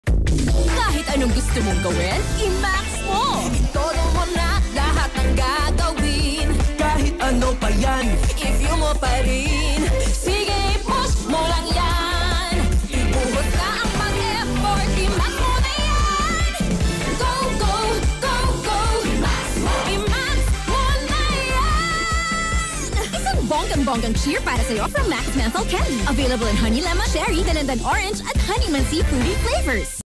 ABS-CBN Shutdown Anniversary Special: MAXX Menthol Candy Radio Commercial 2018-2019 30s
MAXX Fruity Menthol Candy

This Radio Commercial was sourced from DWRR-FM 101.9 MHz My Only Radio For Life Manila.
This Radio Commercial was recorded in December 26, 2019, prior to ABS-CBN Shutdown in May 5, 2020.

DRPS Two Episode No. 158: ABS-CBN Shutdown Anniversary Special: MAXX Menthol Candy Radio Commercial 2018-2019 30s